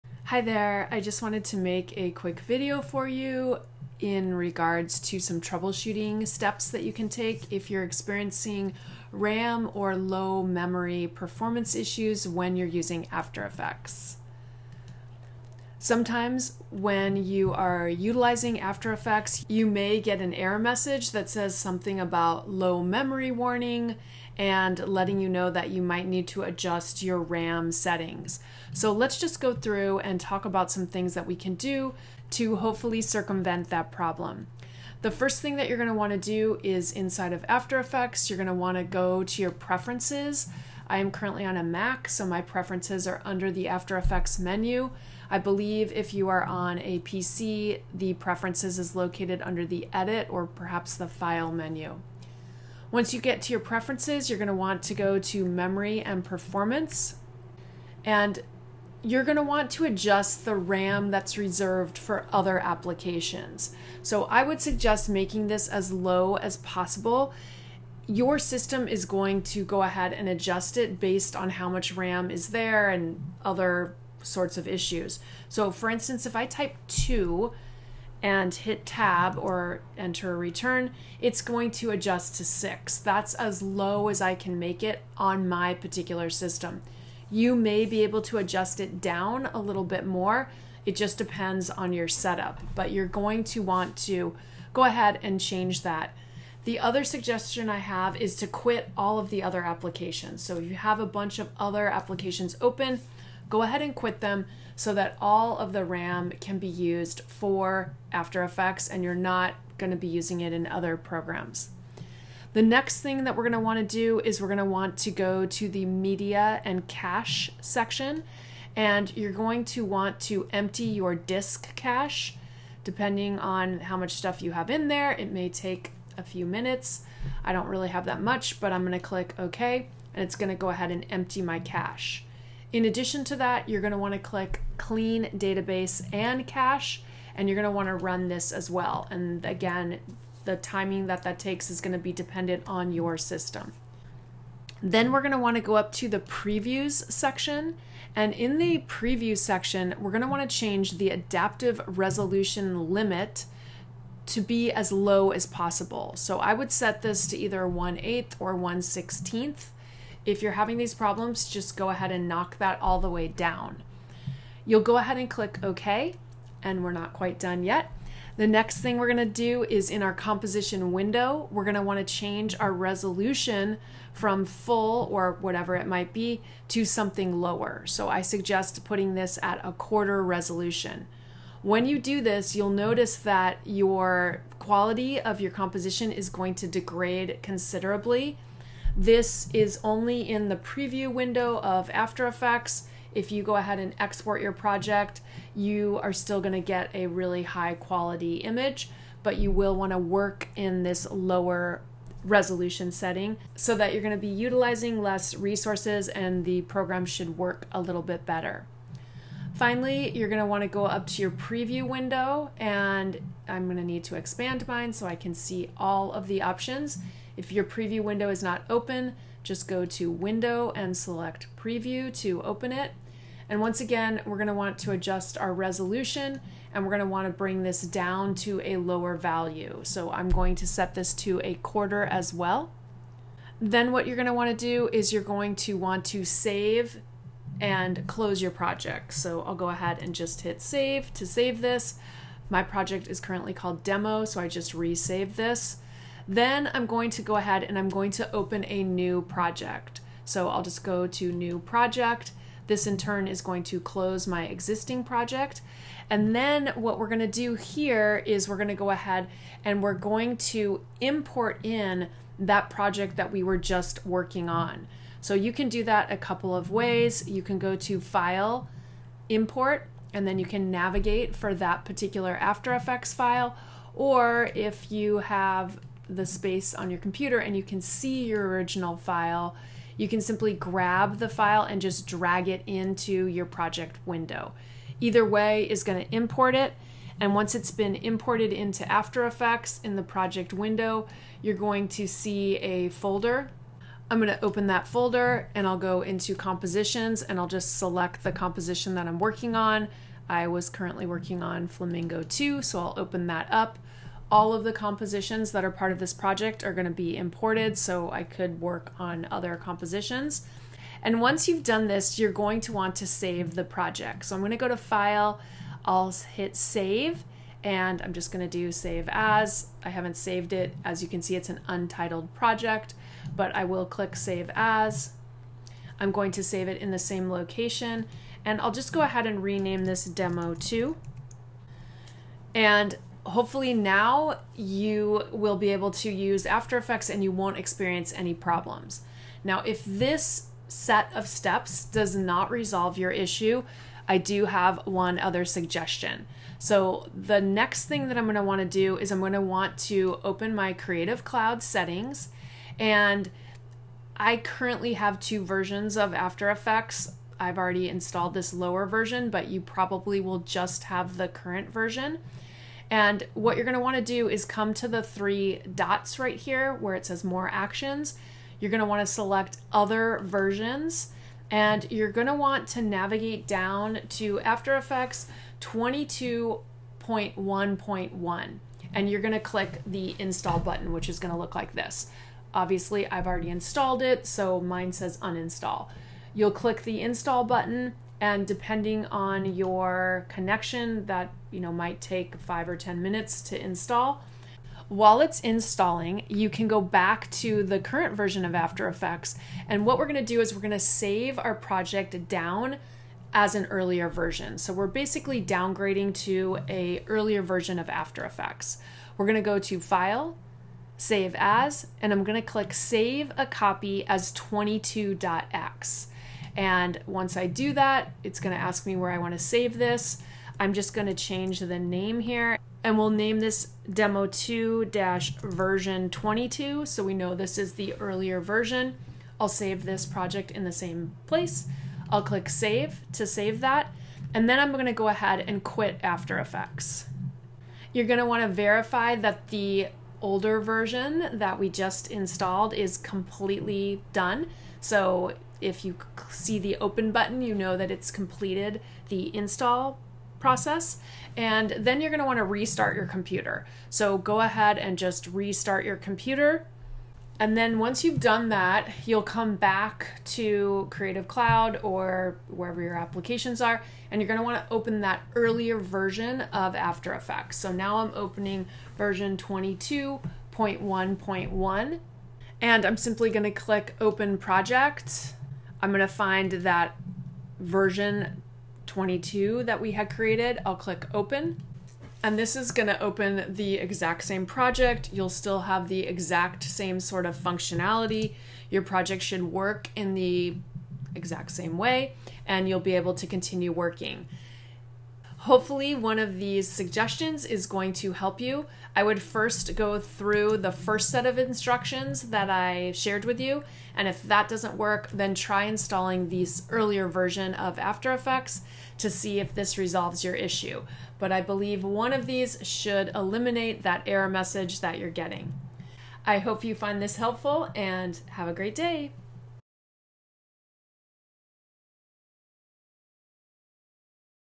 Adobe After Effects | Low memory and/or RAM issues
When encountering the error message "Low memory warning. RAM use has been extended for previews" in After Effects, it indicates that the software is struggling to allocate enough memory to perform the desired tasks, leading to potential performance issues or crashes. In this video I will share with you a few ways to fix this problem and optimize your video editing experience.